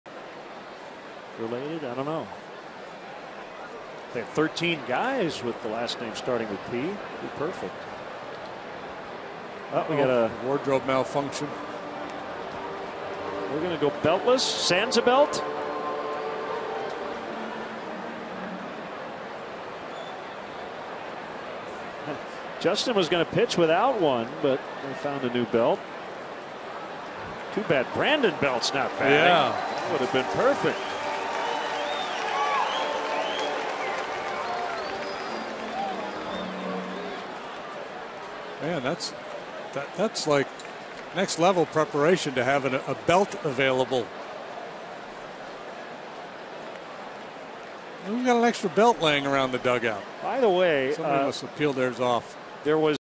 SF@CHC: Grimm changes his belt during the 8th inning
Justin Grimm requires a pause in the action to obtain a new belt during Angel Pagan's at-bat in the top of the 8th inning